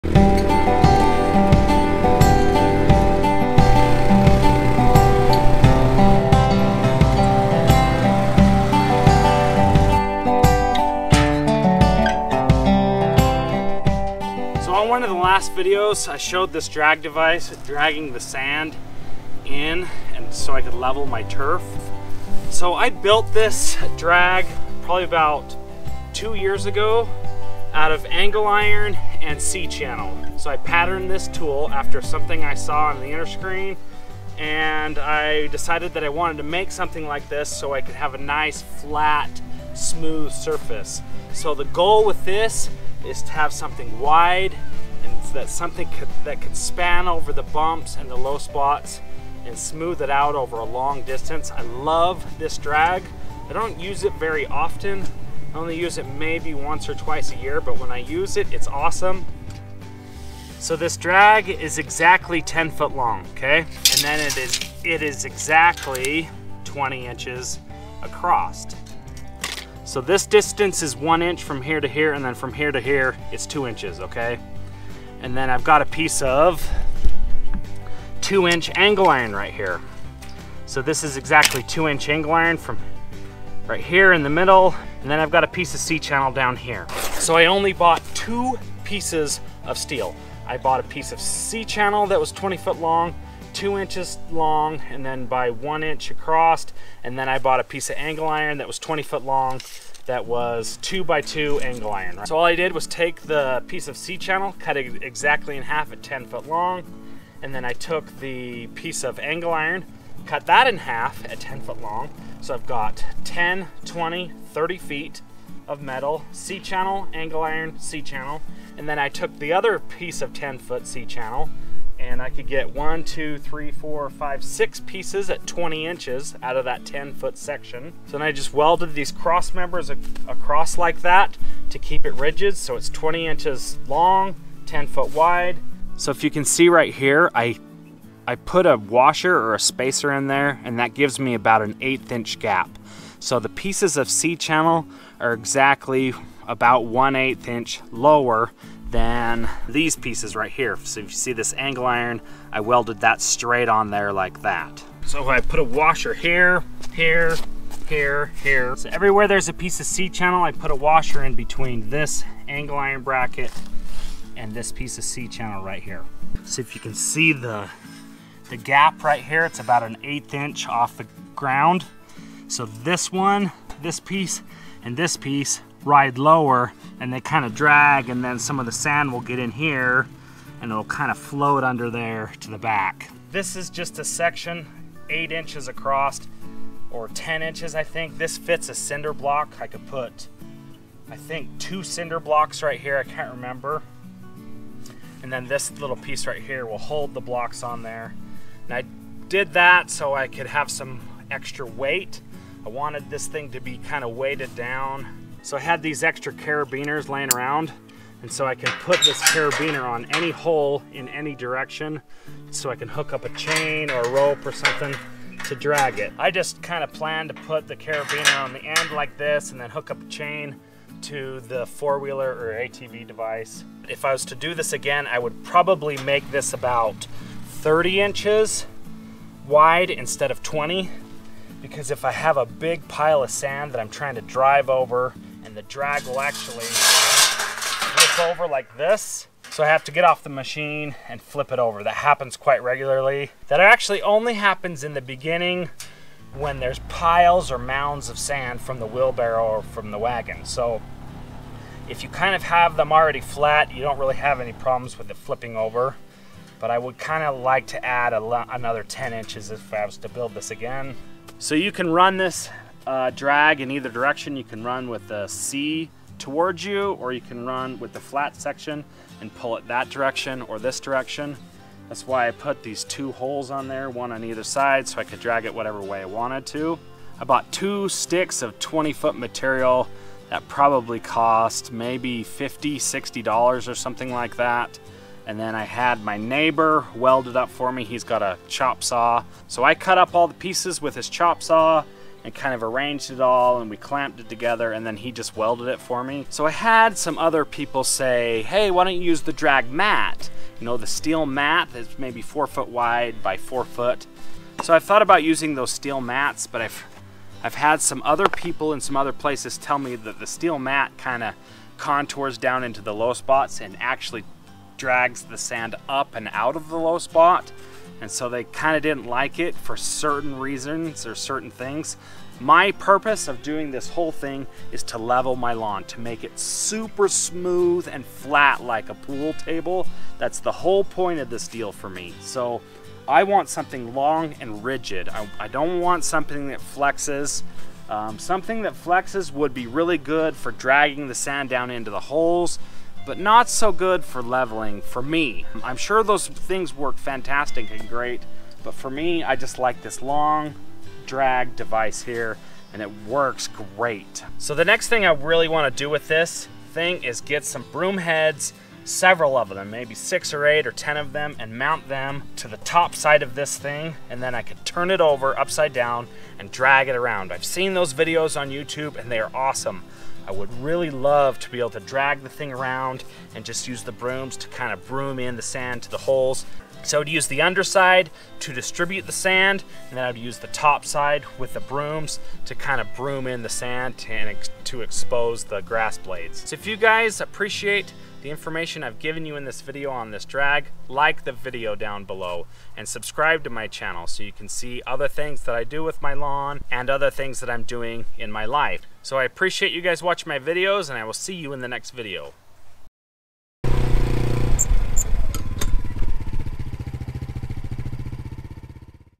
TOP DRESSING drag mat. LAWN LEVELING
This is my top dressing drag mat. I explain how it works and how I built it.

If you want a lawn rebel or domination shirt, I am selling them for $27. This price includes domestic shipping. Sizes and colors are limited. SIZE LARGE LAWN REBEL SHIRTS ARE GONE.